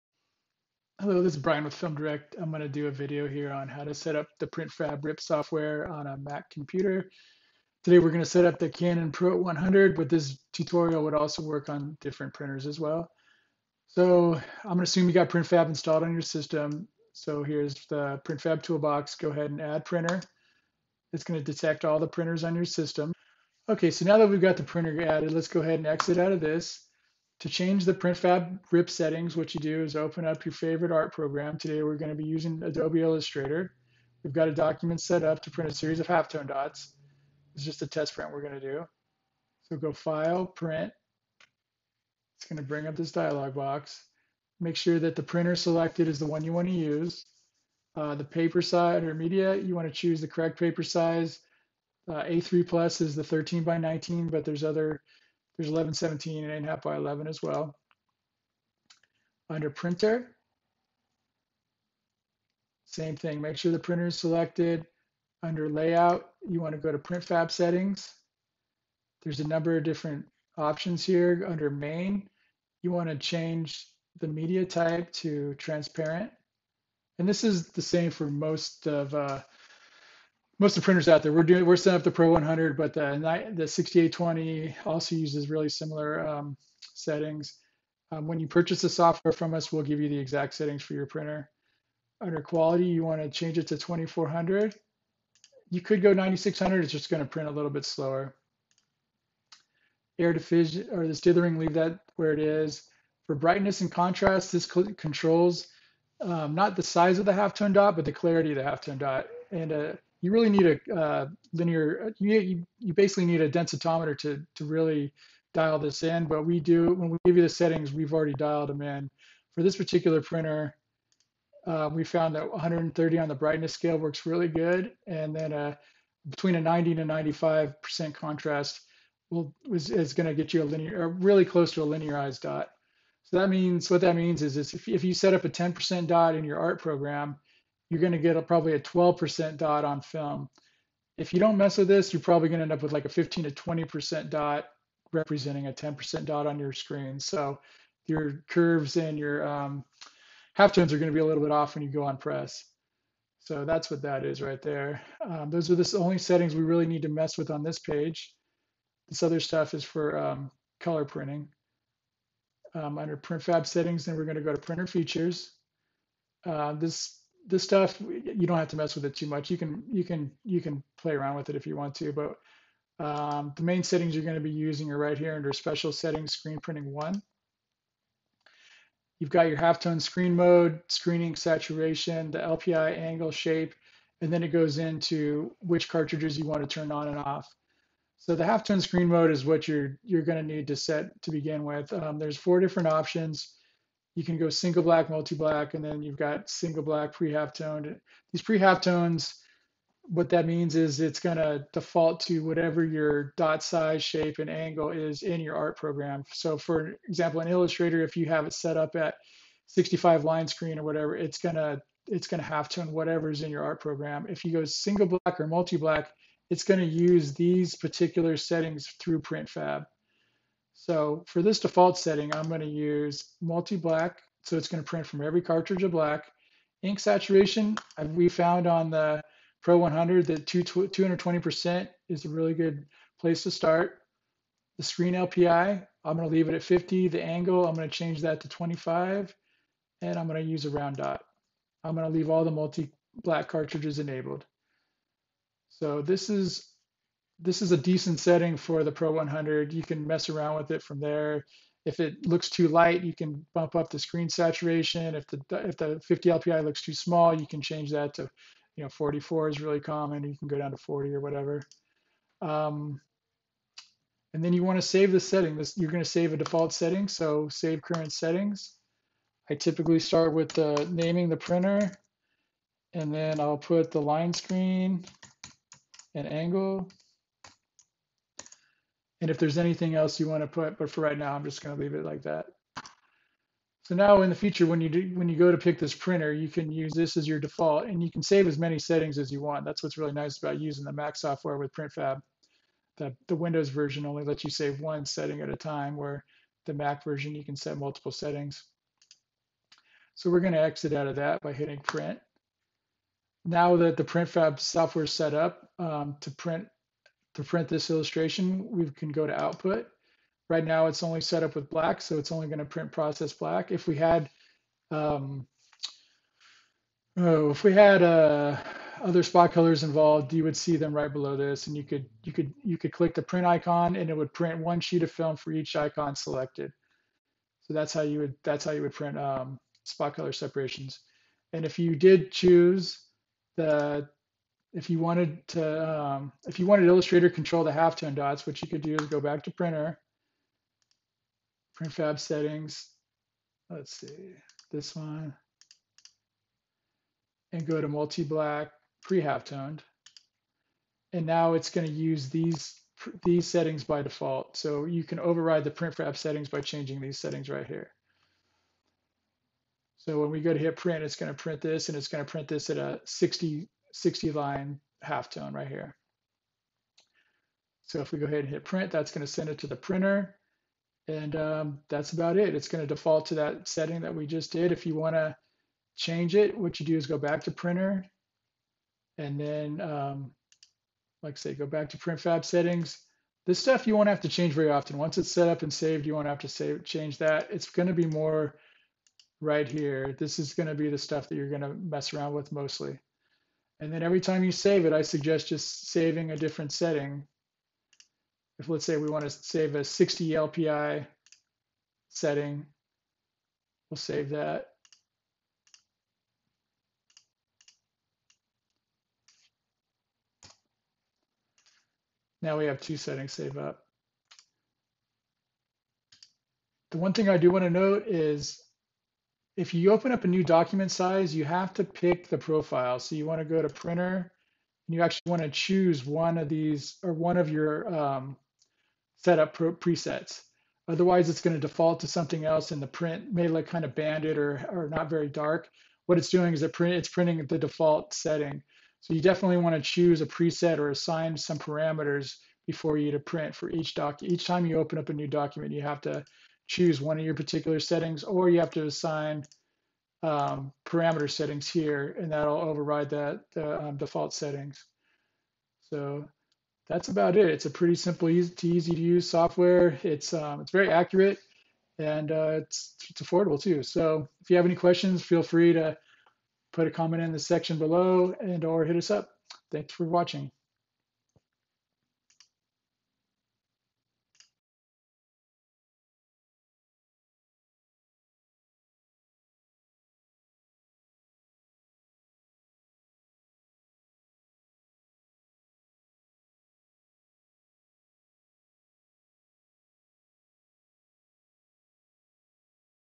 Canon pro-100 with Printfab for Film Output
This video demonstrates how to set-up and use the PrintFab Rip software on a Mac. This video demonstrates the Canon pro-100 but it will work for other printers as well.